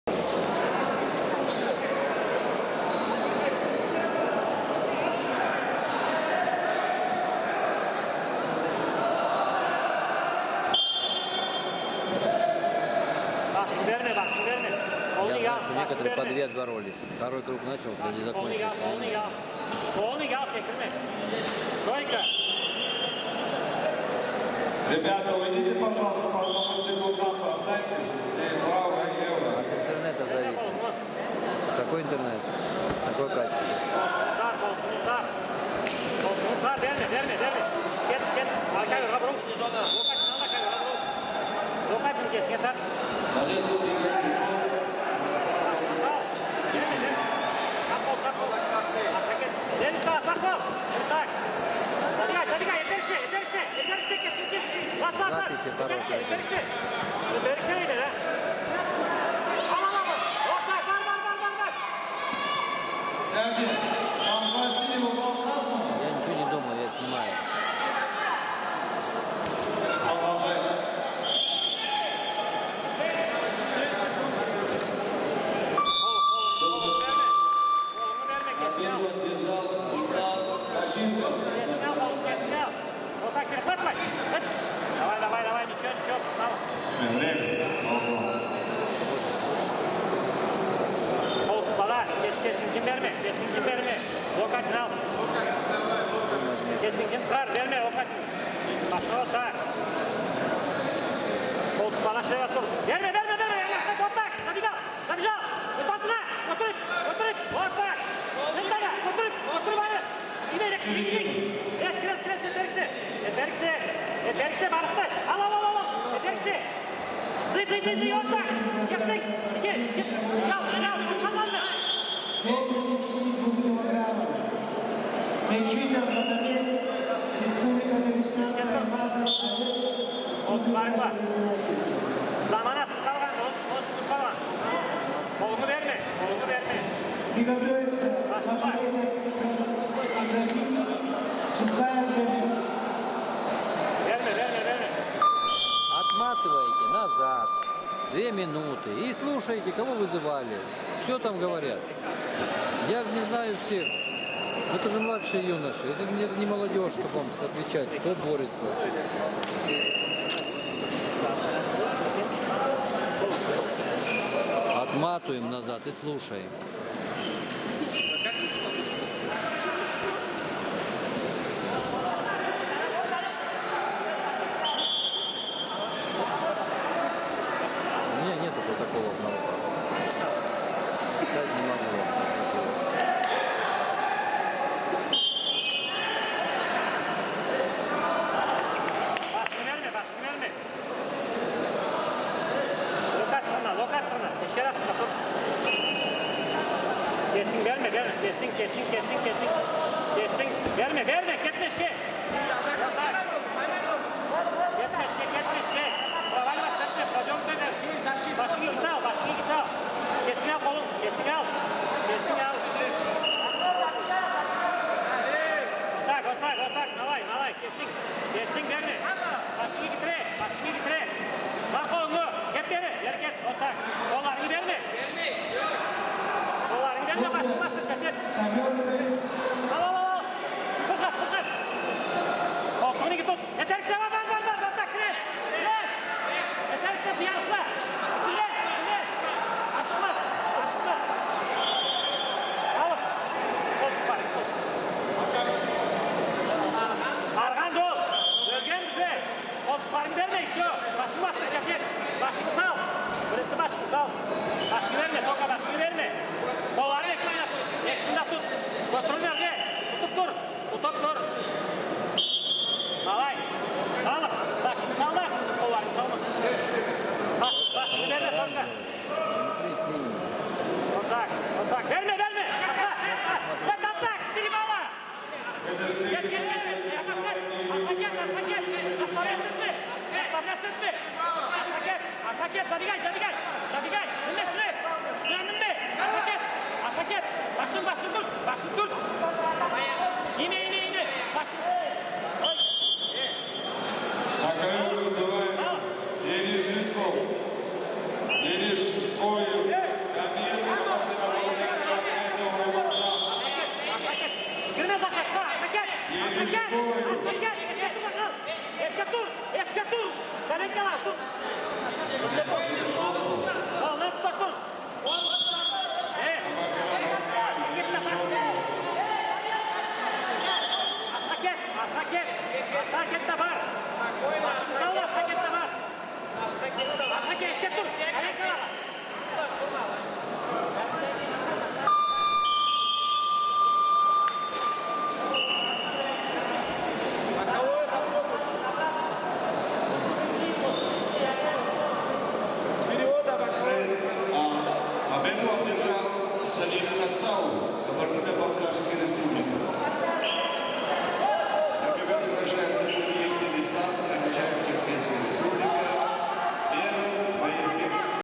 Джангаев Эрик (РСО-Алания) - Хасауов Салих (КБР)
1/16 до 48 кг.
Первенство СКФО 2022 по вольной борьбе среди юниоров до 16 лет (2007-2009 г.р.)
Нальчик.